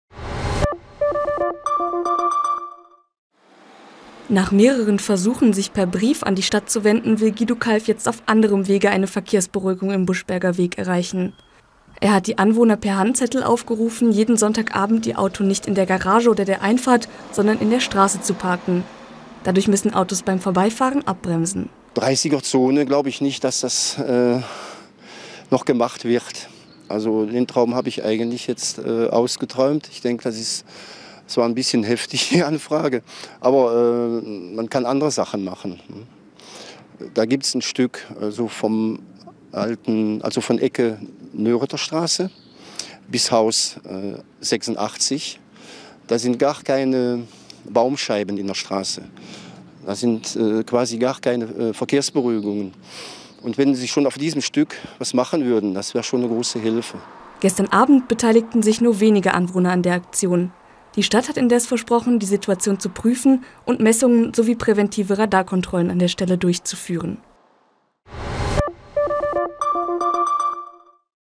Anwohnerinitiative: "Die Buschberger machen sich breit"
"Sind Sie auch der Meinung, dass in unserer Straße zu schnell gefahren wird?", diese Frage stellt Guido Kalf aus dem Buschberger Weg in Kettenis. Mit der Aktion "Die Buschberger machen sich breit" will er weitere Anwohner dazu aufrufen, ein Zeichen für eine Verkehrsberuhigung der Straße zu setzen.